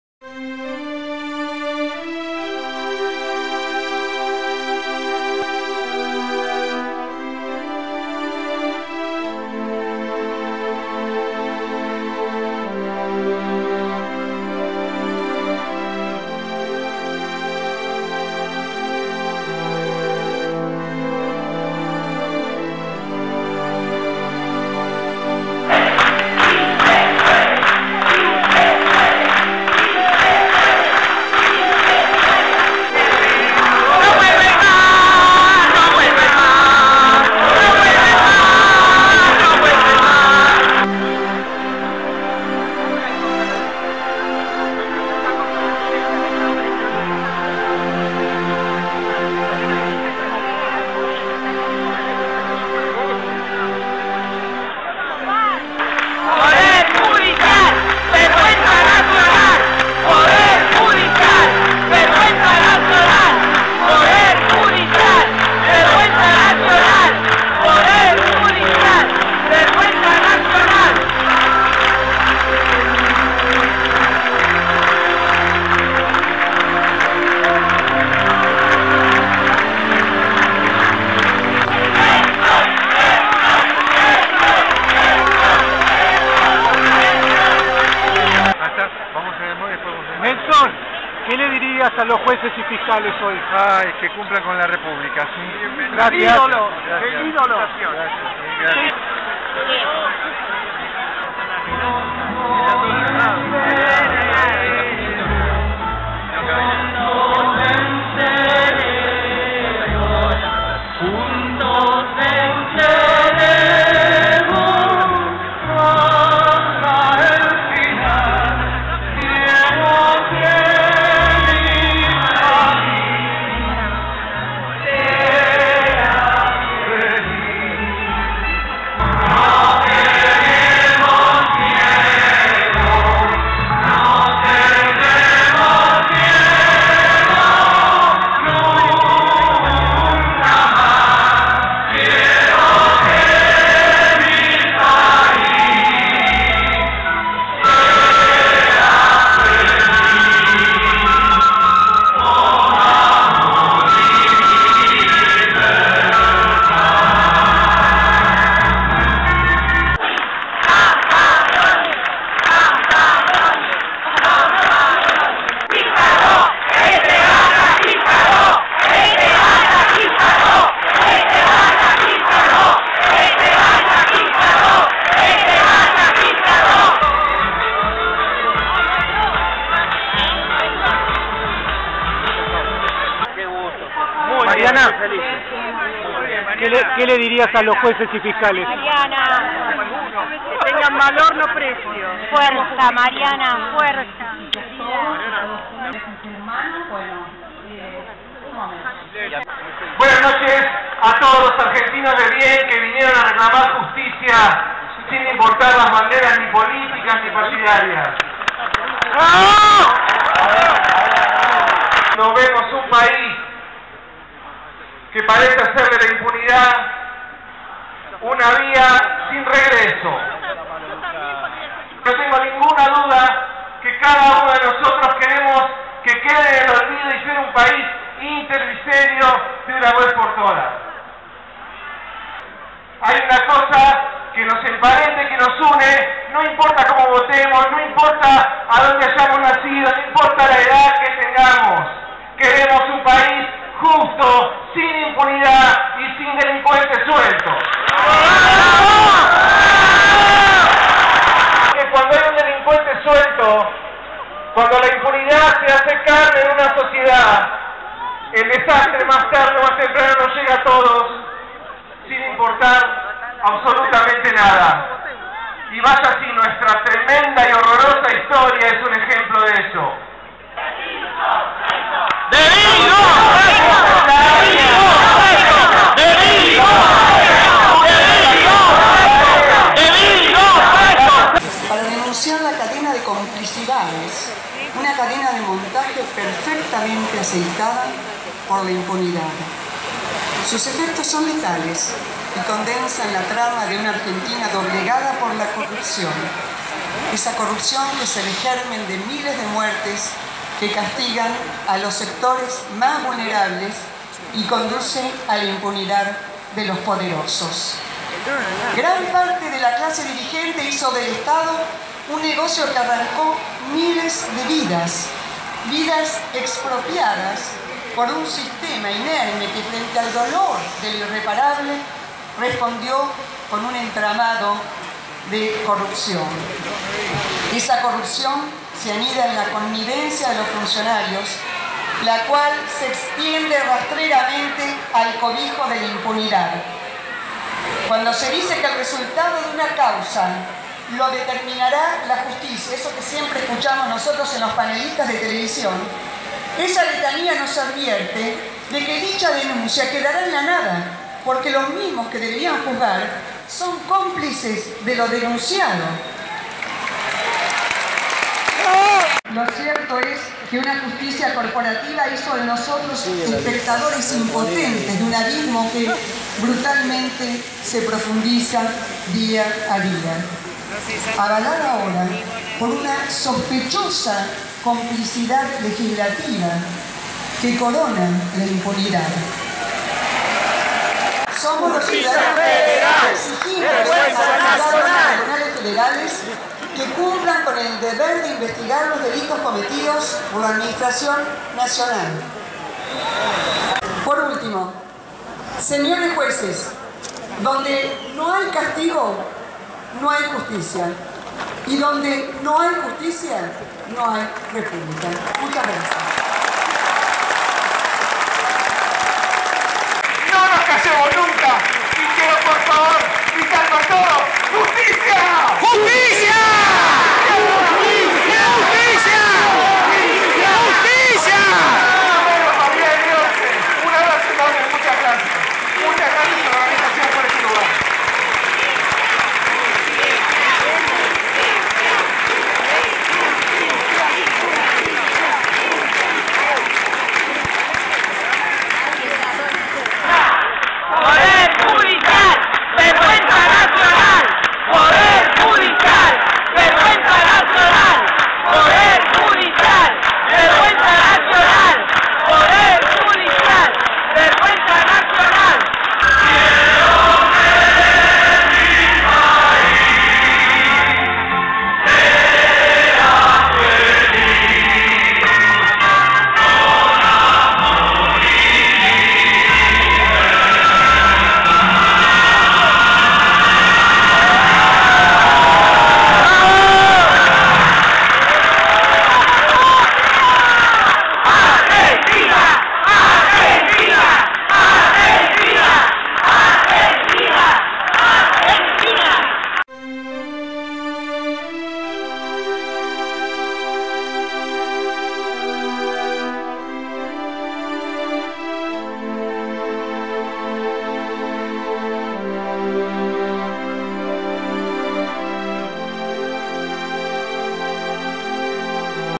Acto del 3 de Agosto 2017 contra la Corrupción y la Impunidad – Síntesis corta
Versión más corta de lo escuchado y vivido en el acto que se llevó a cabo enfrente al palacio de Tribunales en la ciudad de Buenos Aires, en donde se reclamó una acción más efectiva y veloz del Poder Judicial en las causas por corrupción que involucran a funcionarios que aún hoy vergonzosamente mantienen su impunidad, pese a que las innumerables pruebas de los delitos cometidos están a la vista de toda la ciudadanía.

Antes y después de las palabras de los oradores, el resto de los ciudadanos presentes gritamos ¡¡¡ Poder Judicial, vergüenza nacional !!!
Mostramos carteles e hicimos oír nuestras voces manifestando nuestro enojo y reclamo a los Jueces y Fiscales por demorar causas judiciales por motivo del arcaico sistema, por su sometimiento al poder político y/o económico, por sus propias limitaciones, etc. y en algunos casos por ser cómplices al encubrir o declarar inocentes en forma intencional a culpables de corrupción y de otros gravísimos delitos penales.

Te invito a conocer la página web de “Ganamos Todos” ) para ver más contenidos de esta y otras manifestaciones y expresiones de ciudadanos.

La noche del Jueves 3 de Agosto 2017 muchos ciudadanos nos reunimos frente al Palacio de Tribunales, epicentro y emblema del Poder Judicial, para mostrar públicamente nuestro malestar, enojo y disconformidad frente a la tarea de una gran parte de los Jueces y Fiscales miembros del Poder Judicial de la Nación.

Paolo Menghini y Diana Cohen Agrest pusieron sus voces y sus lúcidas reflexiones de ciudadanos frente al micrófono del humilde escenario montado en la esquina de Talcahuano y Lavalle, como dos sufridos padres de víctimas de la impunidad de los responsables y culpables de la muerte de sus respectivos hijos.